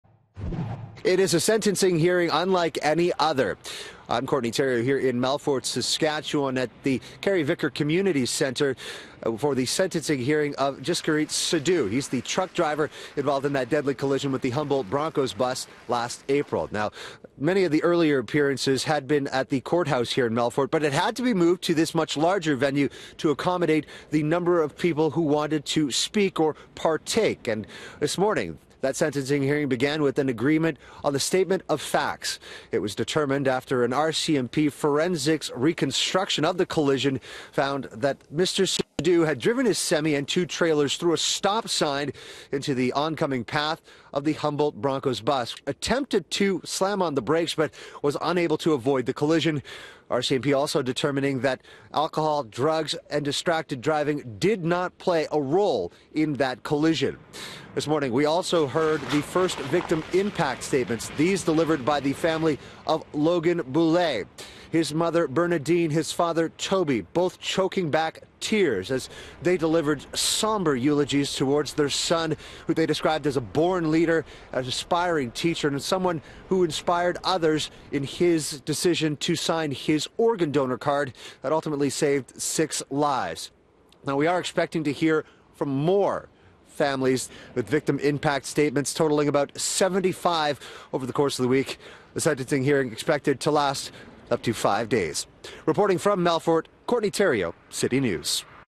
Humboldt victim statements begin at truck driver's sentencing
Several heartbreaking victim impact statements were read by the families of victims from the Humboldt Broncos crash on the first day of truck driver Jaskirat Sidhu’s sentencing hearing. Courtney Theriault has more from the Saskatchewan courtroom.